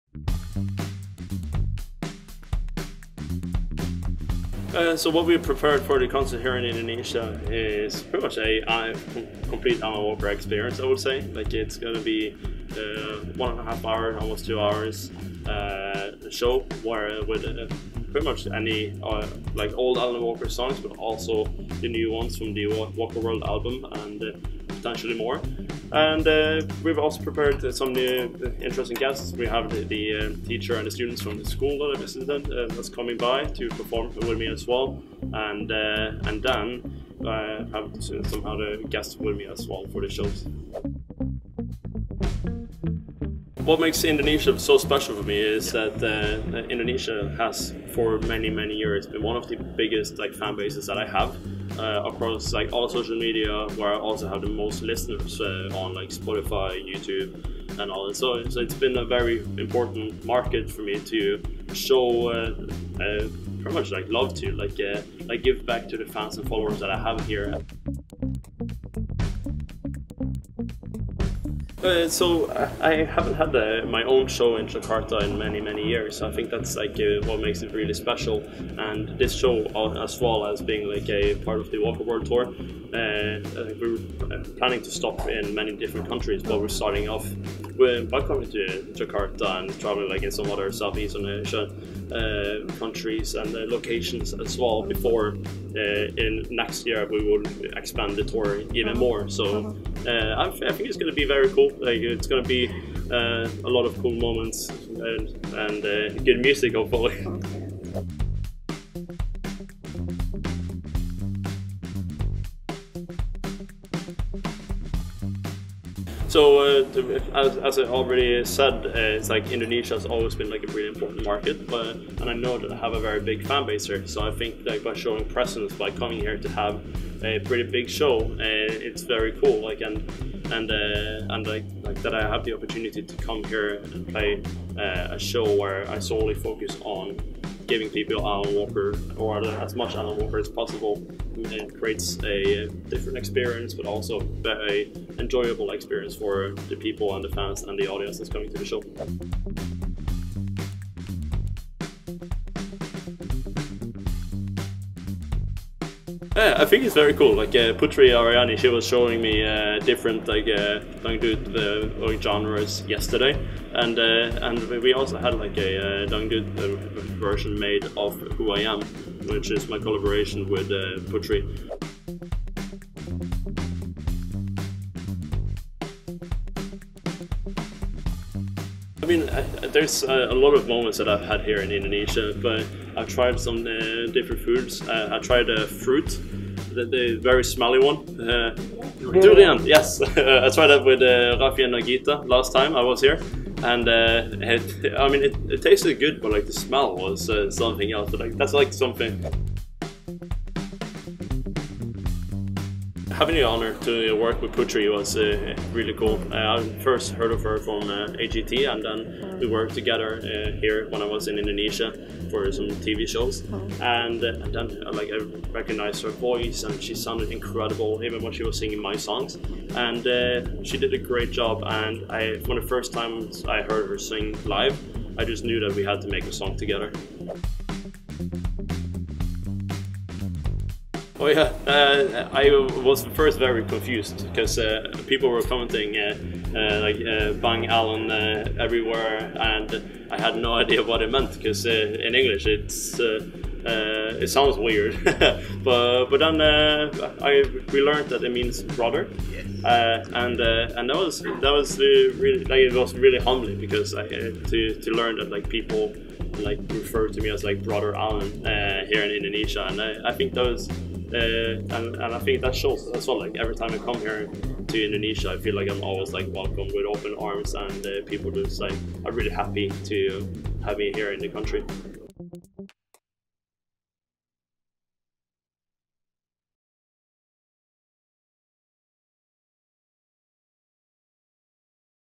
[EKSKLUSIF] Alan Walker Ketagihan ke Indonesia Karena Ini
VIVA - Alan Walker akan segera bertemu dengan penggemar di Indonesia lewat konser bertajuk Walker World pada 8 Juni 2024 mendatang. Konser ini bakal diselenggarakan di Phantom Pantai Indah Kapuk 2.

Seminggu sebelum konser diadakan, Alan Walker sudah tiba di Indonesia untuk melakukan banyak persiapan. Termasuk mendatangi guru dan siswa di sekolah SMA Swasta Al-Azhar Medan.

[DRP-DA] {NAT}

Reporter : Rizkya Fajarani Bahar
Kontributor : -